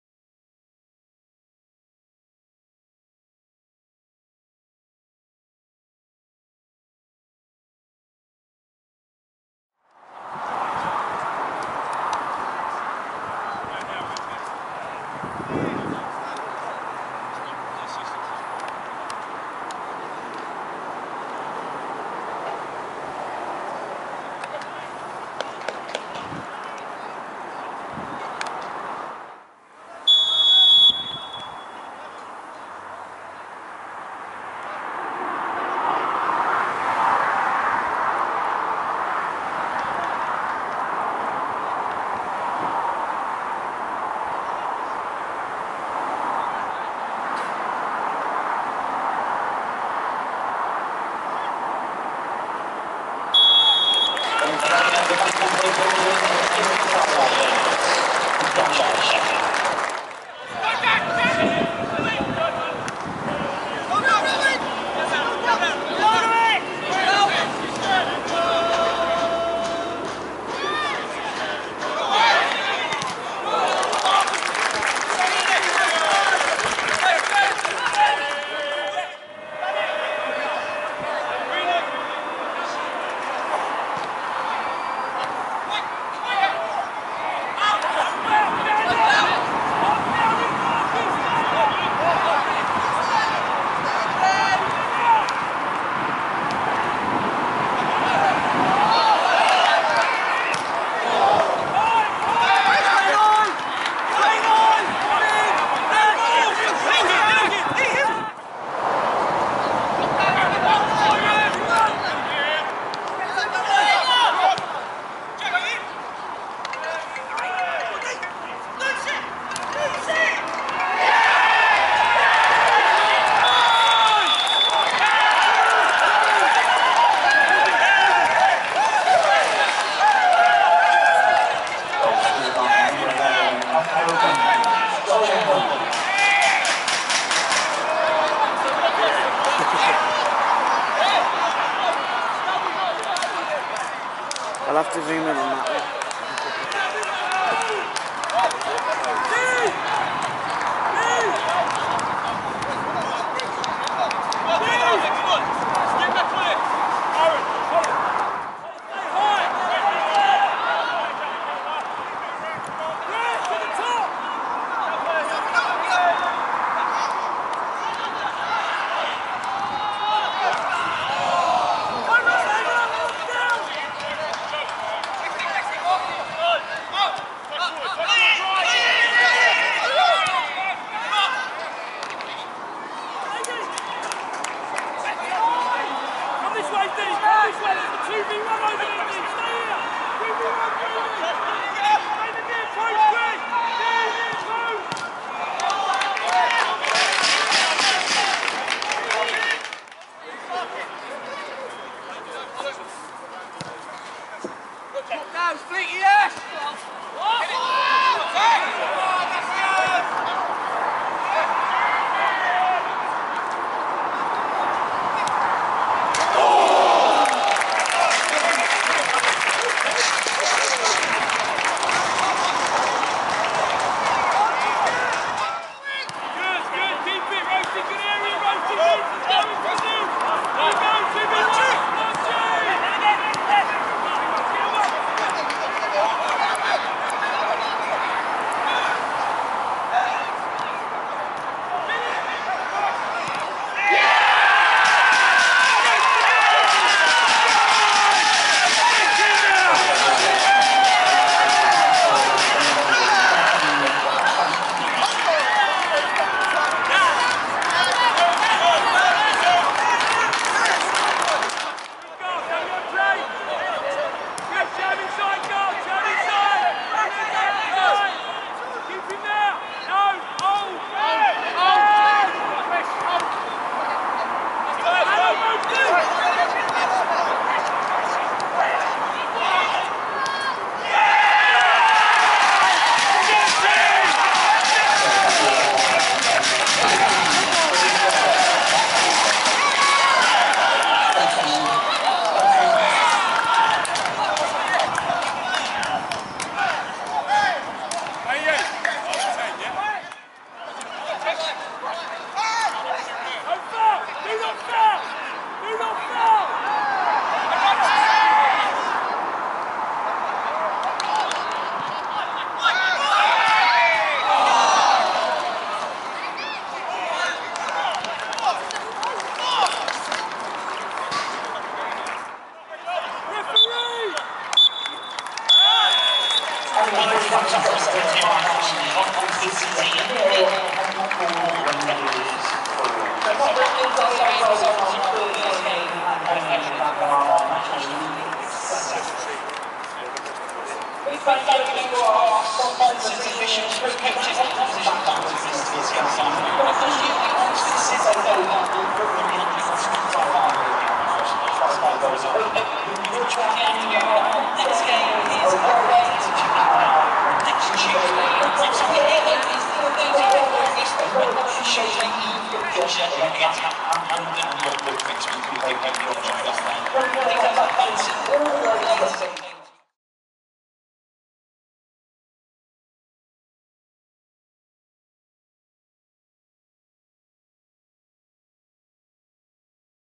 OXFORD CITY V CONCORD RANGERS FC - VANARAMA NATIONAL LEAGUE SOUTH HIGHLIGHTS
All the preperations for the big kick off of the 2019/2020 Vanarama National League South Season was all in vein as Oxford City were left humbled on the opening day as Concord Rangers put three unanswered goals past them at Marsh Lane on Saturday afternoon.

Joel Nouble, Kreshnic Krasniqi and Joe Gardner made sure that Danny Scopes' men headed back to Canvey Island with all 3 points.

There was also a minutes silence held in memory of long serving club secretary and club legend, John Sheppard, also known as Mr Oxford City FC through his many years of service to the club.